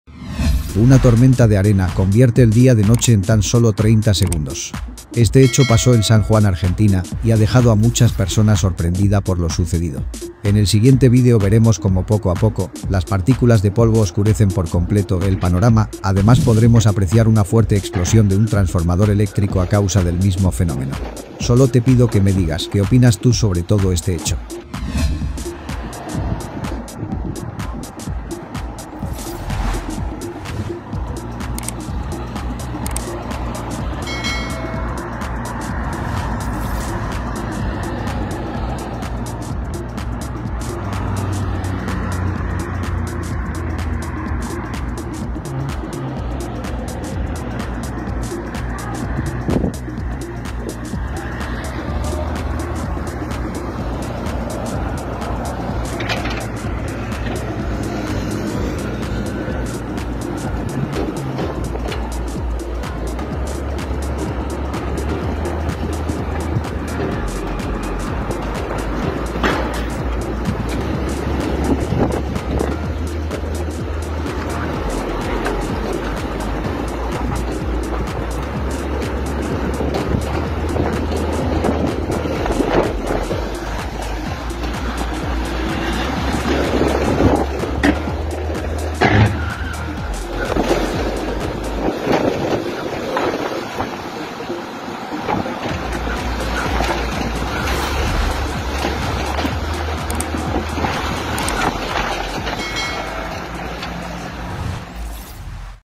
TORMENTA de ARENA azota ARGENTINA PASO del DIA a la NOCHE EN 30 SEGUNDO en SAN JUAN ARGENTINA
PASO del DIA a la NOCHE EN 30 SEGUNDO en SAN JUAN ARGENTINA TORMENTA de ARENA azota ARGENTINA
  Una tormenta de arena vuelve el día de noche en tan sólo 30 segundos. este hecho pasó en san juan argentina, y ha dejado a muchas personas sorprendida por lo sucedido. En el siguiente vídeo veremos como poco a poco, las partículas de polvo oscurecen por completo el panorama, además podremos apreciar una fuerte explosión de un transformador eléctrico a causa del mismo fenómeno.  solo te pido que me digas que opinas tú sobre todo este hecho.